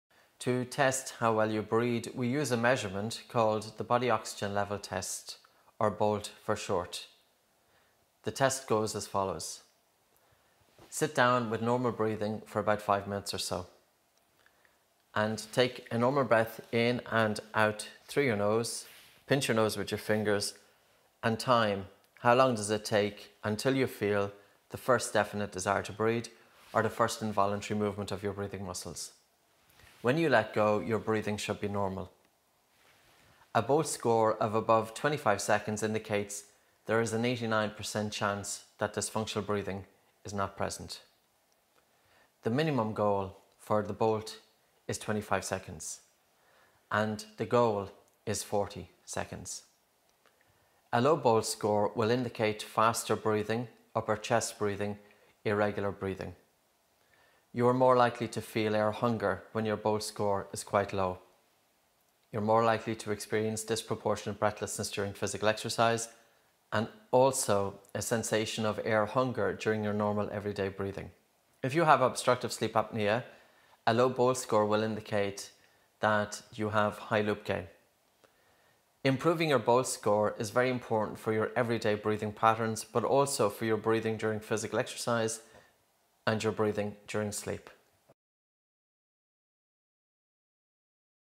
What is BOLT Score?
The Body Oxygen Level Test, or BOLT for short is used to test how well we breathe. To start the test, sit down and engage in your normal breathing for around five minutes. Take a normal breath in and out through your nose. Then pinch your nose with your fingers and time how long it takes until you feel the first desire to breathe or the first involuntary movement of your breathing muscles.

When you let go your breathing should be normal. A BOLT score above 25 seconds indicates there is an 89% chance dysfunctional breathing is not occurring. This is the minimum goal for the BOLT and the main aim is to get to 40 seconds. Low BOLT scores indicate faster breathing, irregular breathing, and upper chest breathing. For your everyday breathing patterns and breathing during sleep and physical activity, it is vital to improve your BOLT score.